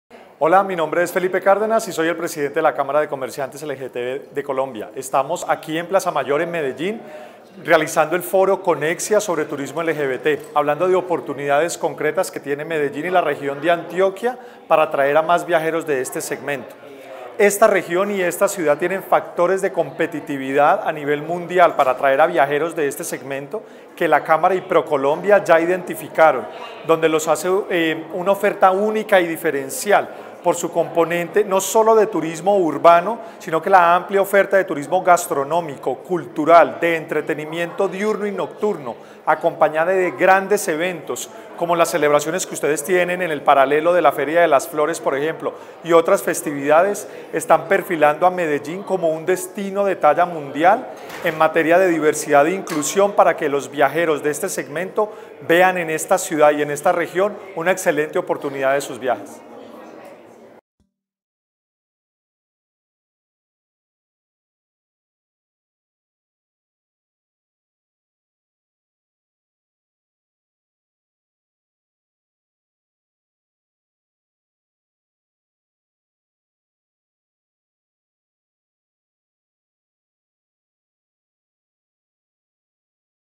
Felipe Cardenas Presidente de la Camara de Comerciantes LGBT
Apoyamos el Foro Conexía, organizado por la Cámara de Comerciantes LGBT de Colombia, en el cual se dieron a conocer las oportunidades para consolidar a Medellín como un destino Gay Friendly. El segmento LGBT (lésbico, gay, bisexual, transgénero) presenta grandes oportunidades para la industria turística: según la Asociación Internacional de Viajes para Gays y Lesbianas, esta población gasta hasta un 40% más en turismo y viajan hasta 3.8 veces más que el resto de las personas.